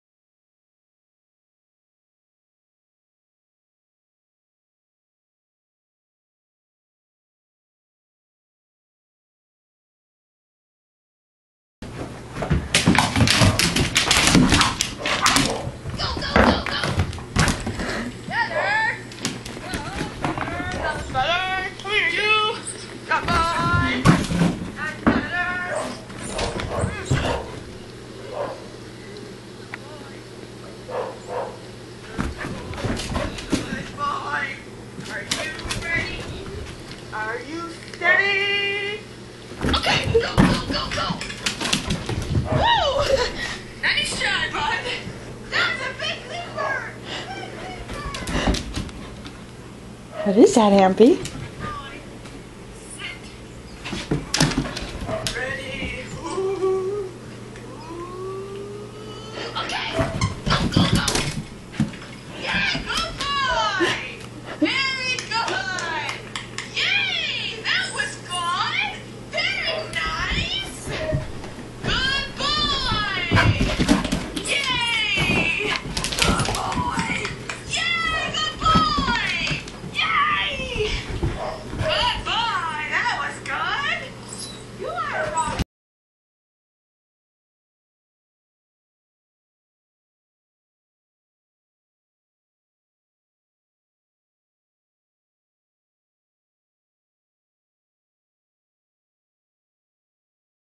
AMP Hears Noises
This was from today while I was editing my running contact video!

Not sure what I would do without AMP =)